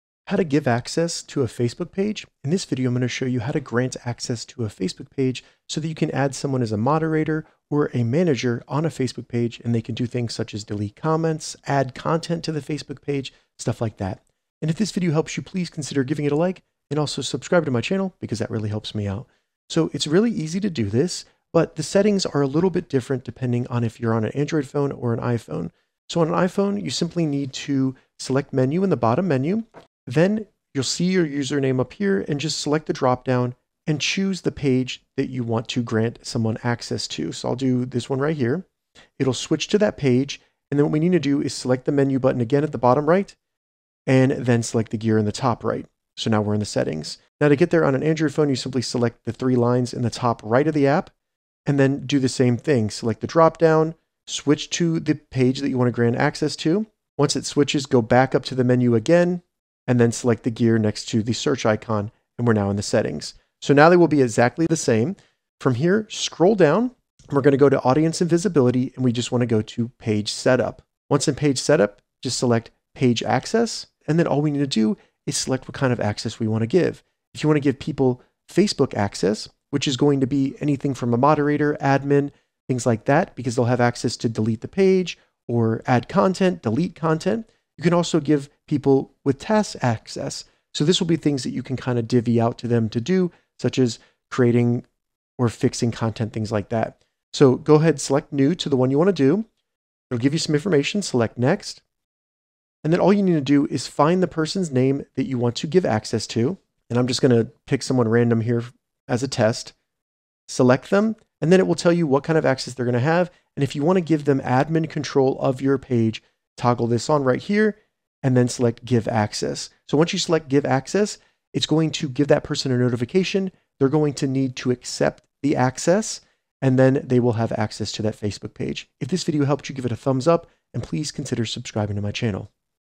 How to Give Access to Facebook Page - iPhone & Android
How to Give Access to a Facebook Page in 2025. In this tutorial, I’ll show you step by step how to add someone as an admin, editor, or moderator to your Facebook Page using the latest settings on desktop and mobile. This means you can safely share control of your page with team members, business partners, or social media managers without sharing your personal login.